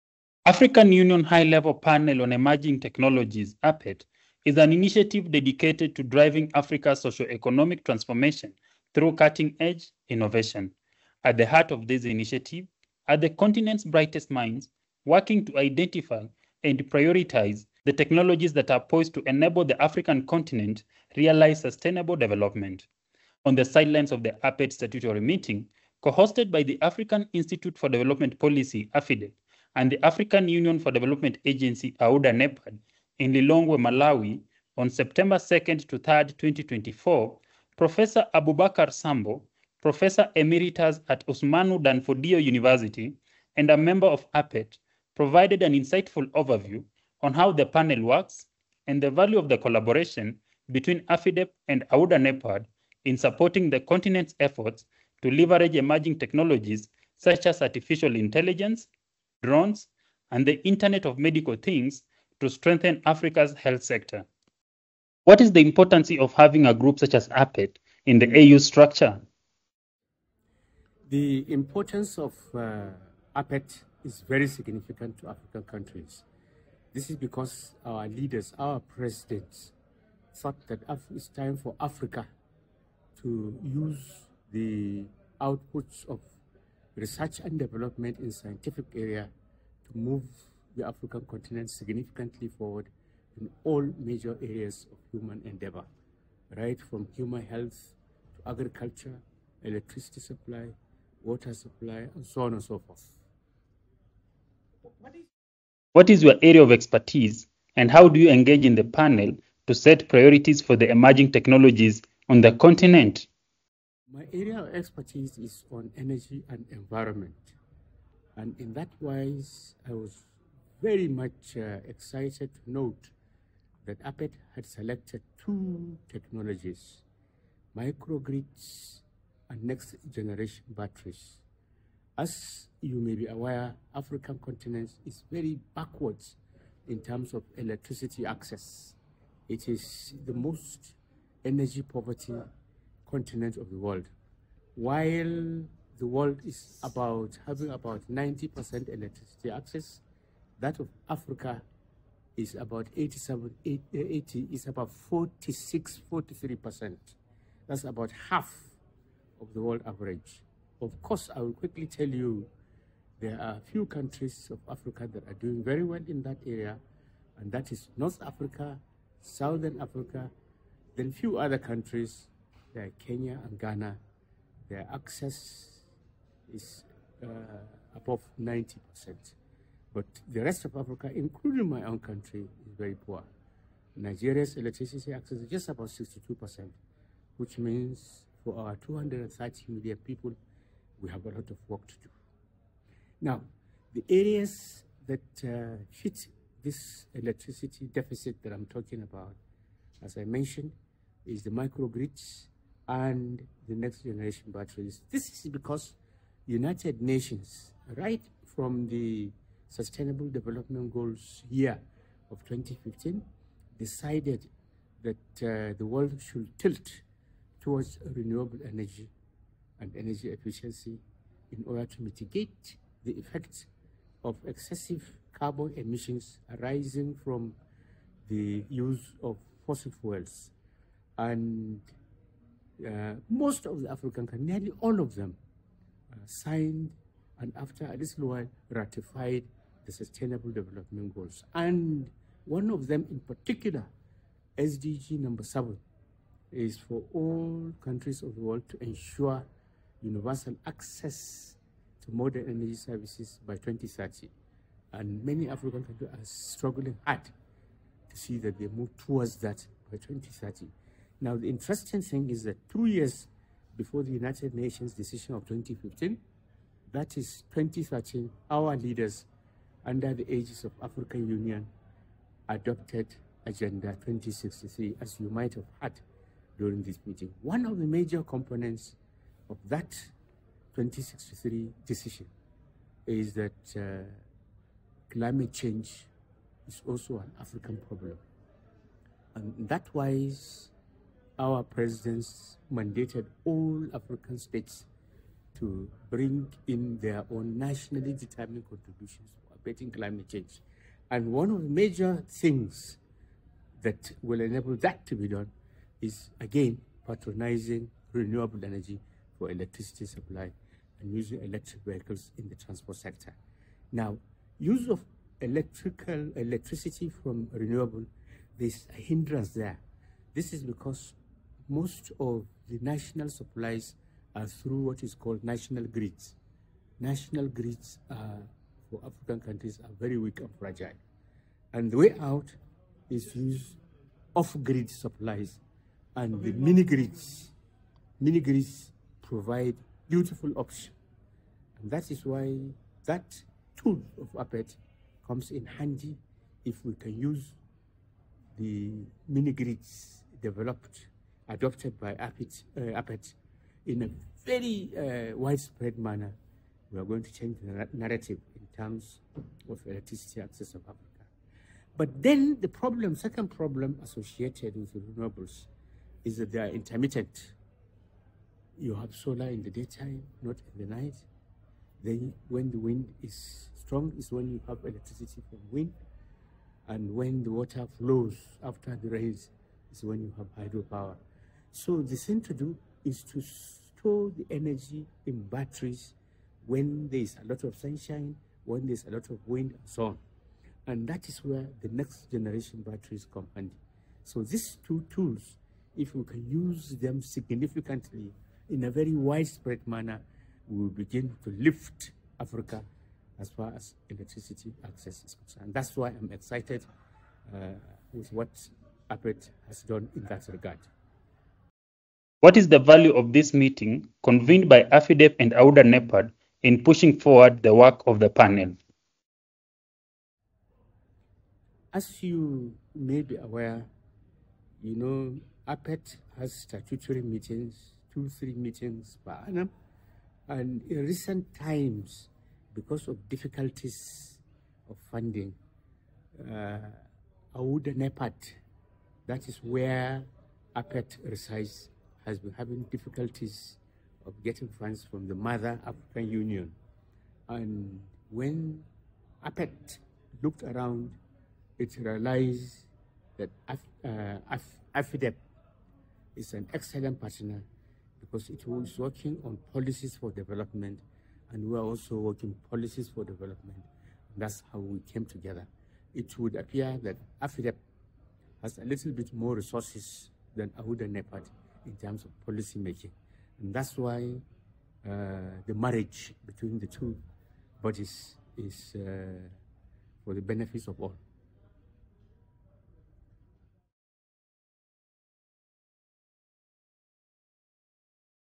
Prof. Abubakar Sambo on why emerging technologies are key for Africa
At the second statutory meeting of the AU High-Level Panel on Emerging Technologies (APET) in Malawi, hosted by AFIDEP and AUDA-NEPAD, Prof. Abubakar Sambo, Professor Emeritus at Usmanu Danfodiyo University and a member of APET, shared insights on how Africa can use technologies like mini-grids and next-generation batteries to address climate change.